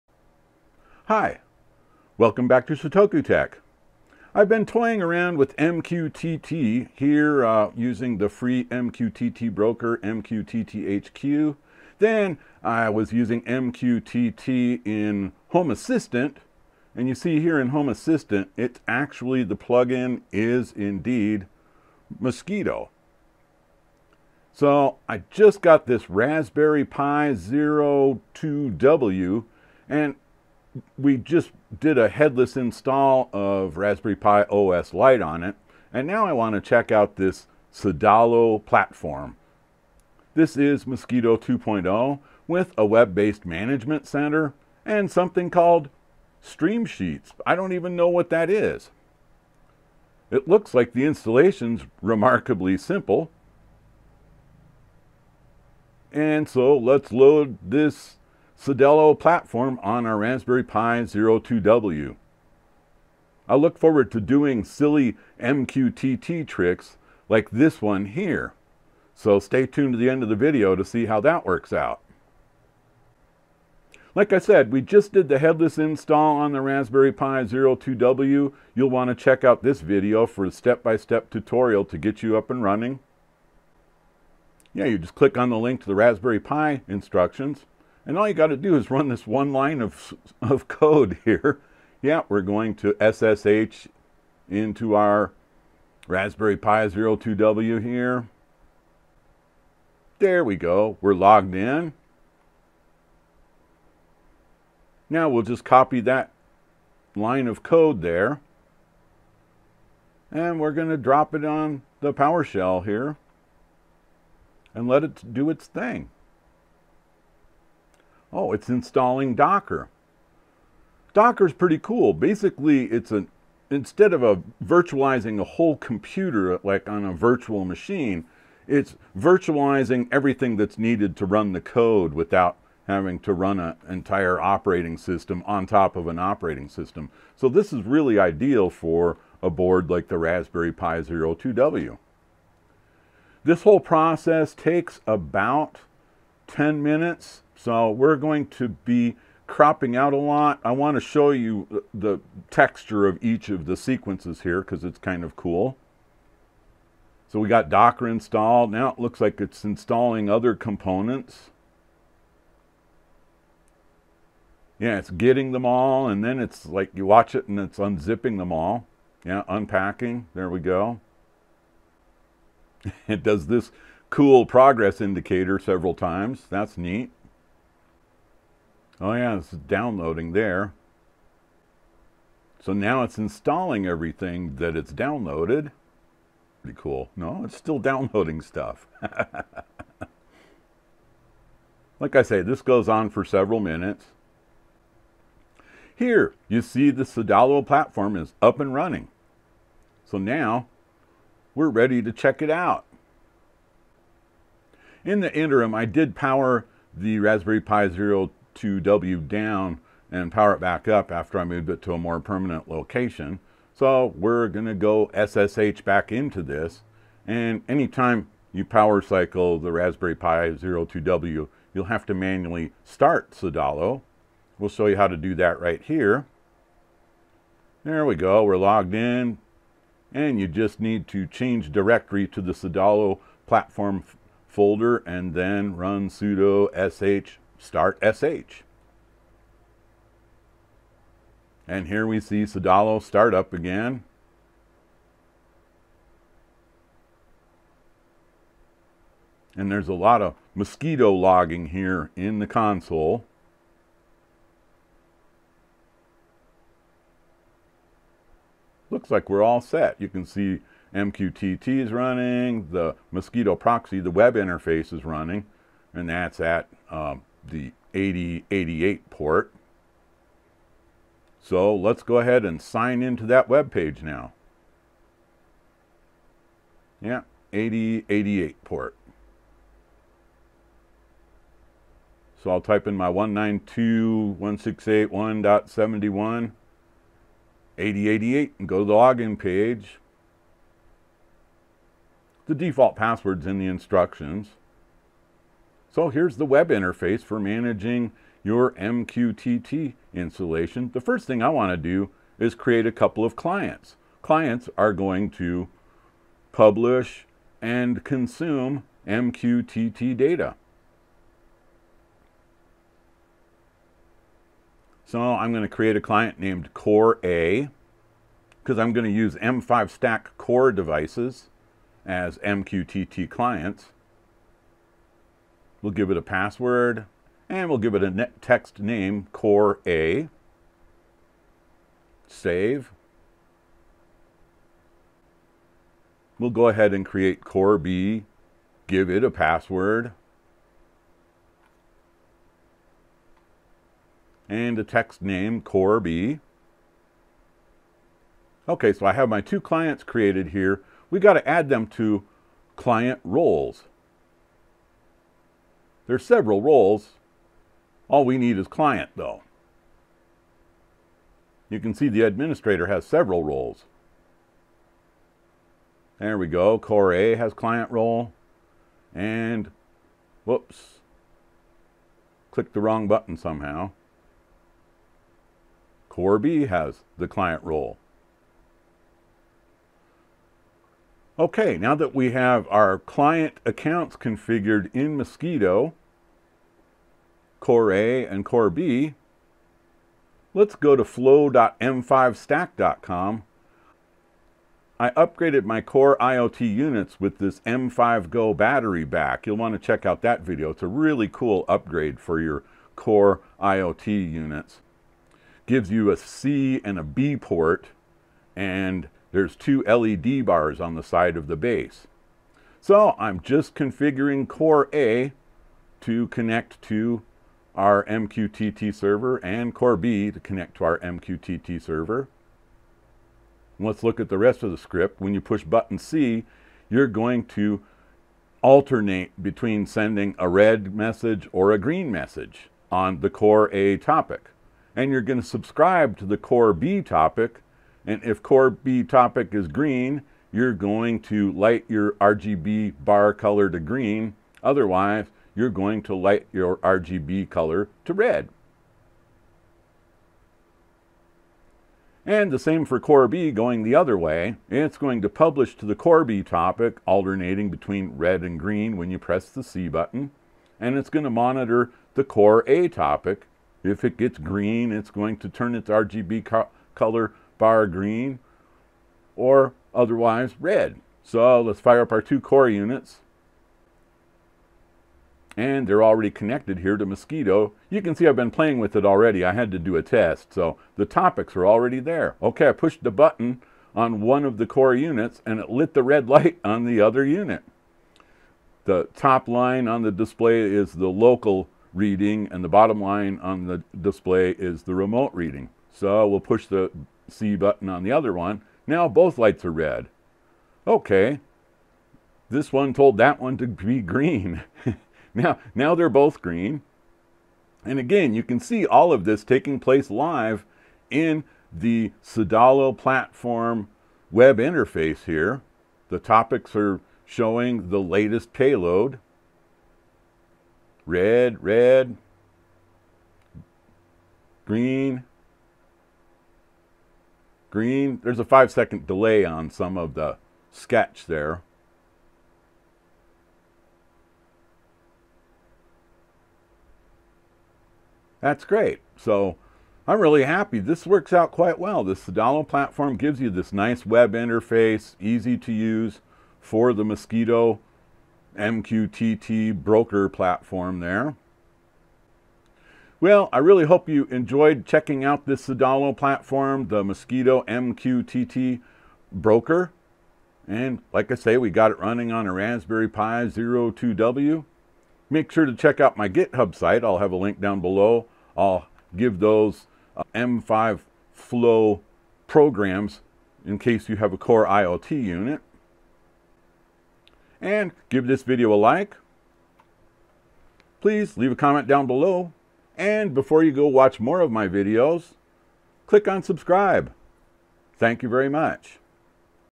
Mosquitto MQTT Broker on Raspberry Pi Zero 2 W Using Cedalo Platform
In my previous video we installed Raspberry Pi OS Lite on the new Raspberry Pi Zero 2 W headlessly, no keyboard or monitor. Now, I want to build my own Mosquitto MQTT broker. This Cedalo Platform installs Mosquitto MQTT broker, a Web Management Interface and something called Streamsheets. It is so simple to just copy and paste one line of code into an SSH session in PowerShell to install Docker and this Cedalo Platform container. Watch step by step installation. Configure clients in the management center. Then we program a pair of M5Stack Basic Core IoT units to publish and subscribe to dualing topics using UIFlow (flow.m5stack.com). The Core units then turn each other's RGB LED bars red or green based on a button pressed on each of the Core units.